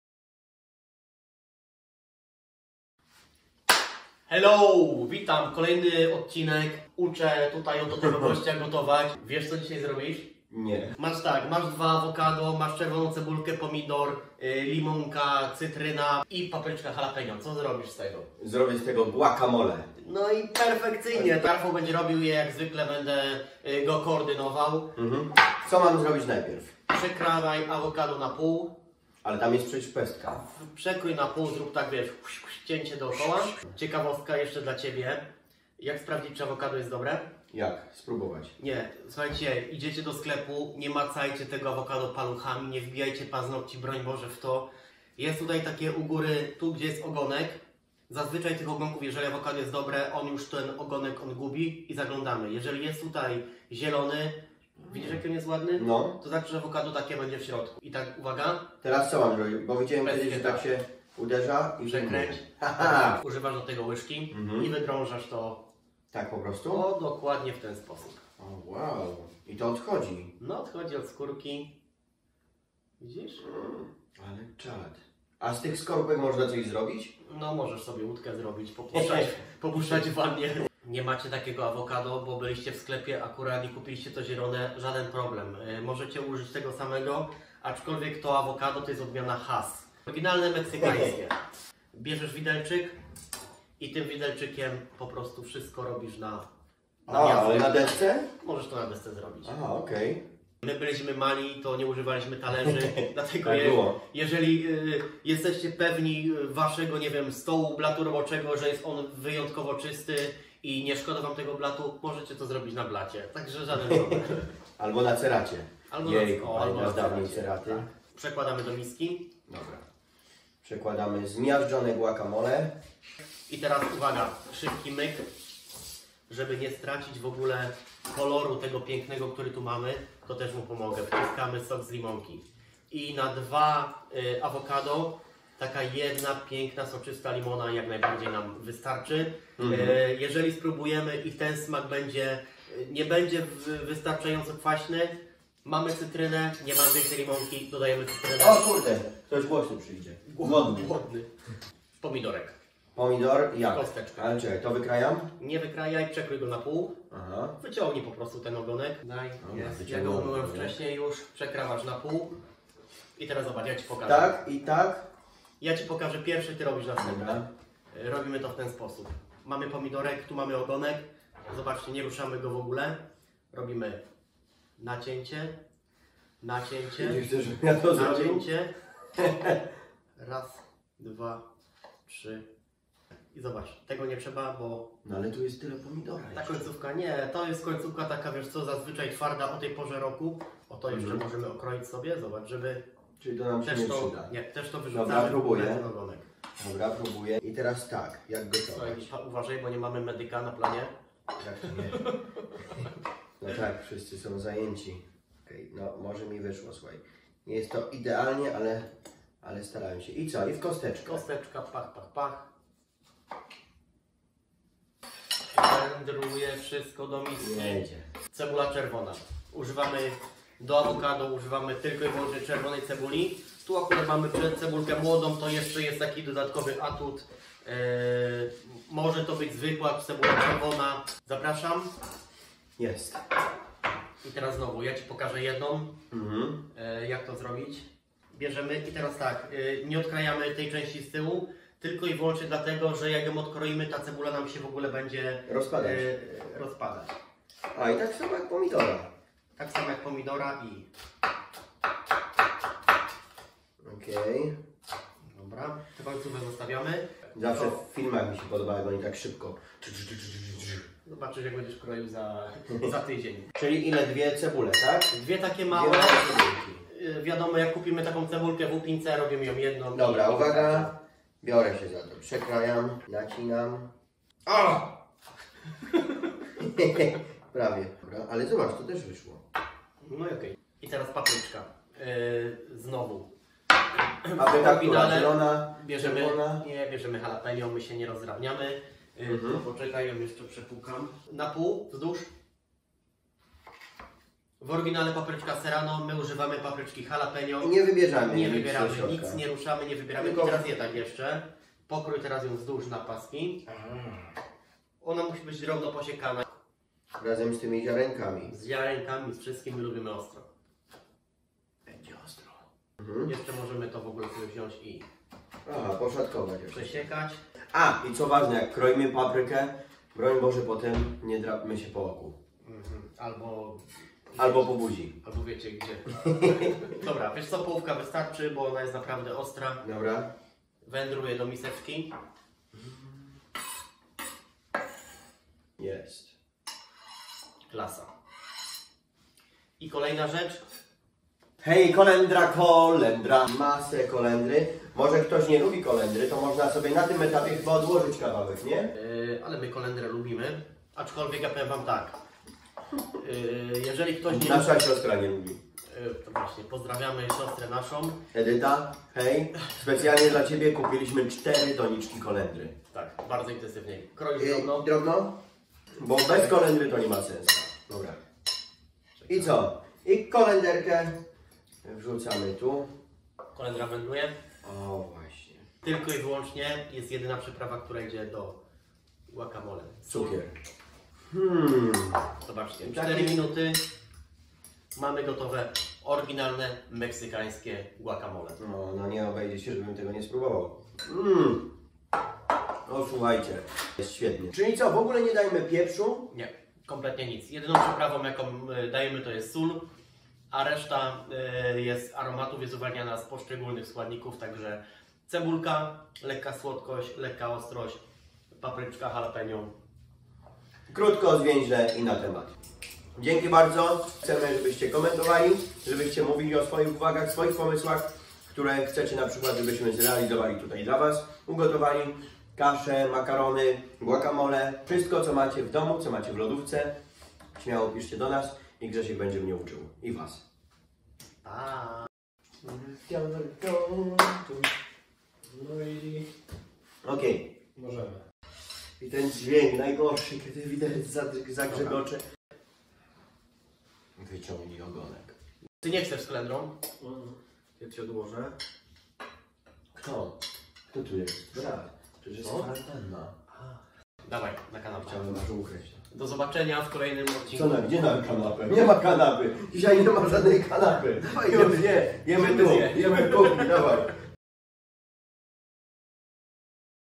Zawierucha w kuchni - guacamole oryginalne meksykańskie
Chcecie zobaczyć jak robi się guacamole w Meksyku? Zrobimy to w naszej kuchni. Ten przepis to autentyczne danie. Robiąc tak mamy pewność, że żaden Meksykanin się na nas nie obrazi:)))